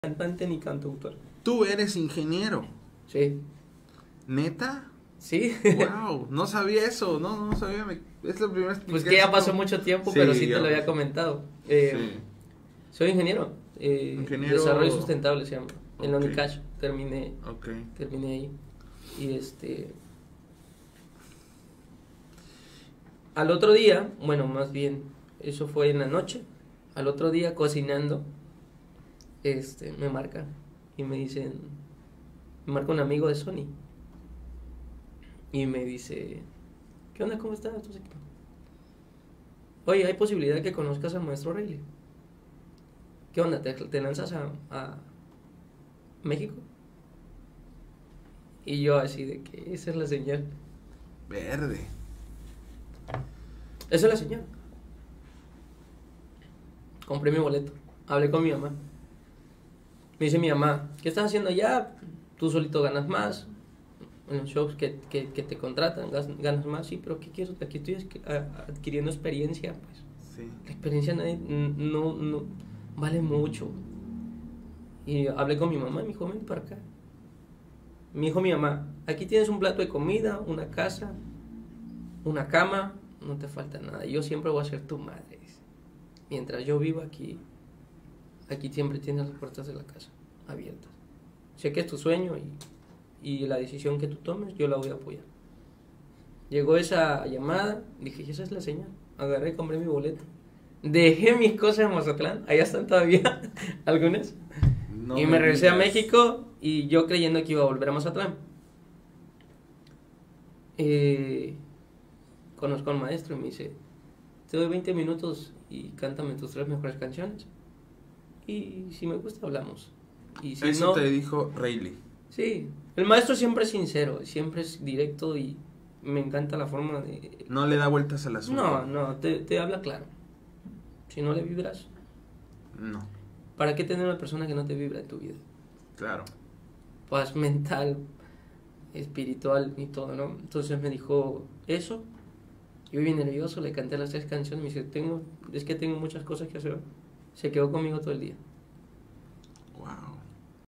Así conocí a Reyli Barba ￼￼
El cantautor Alexis Cristóbal narra el momento de cómo fue que conoció a Reyli Barba ￼